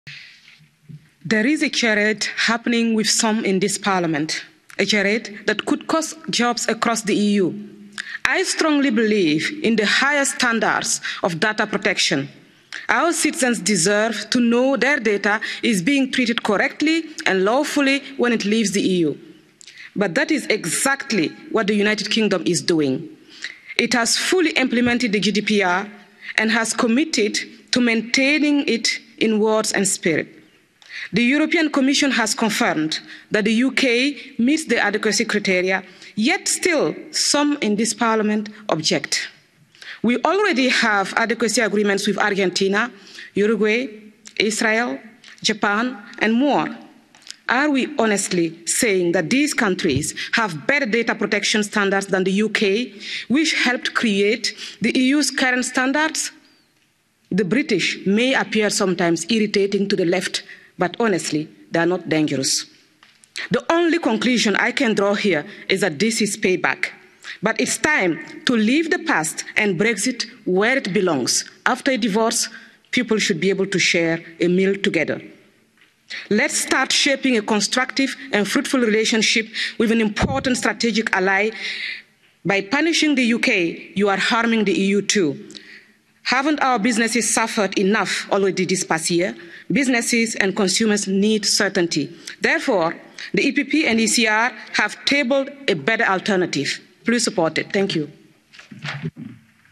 "By punishing the UK, you are harming the EU too." | MEP Assita Kanko
ECR MEP Assita Kanko calls on the European Parliament to leave Brexit in the past, and to start shaping a constructive, fruitful relationship with the UK – an important strategic ally to the EU.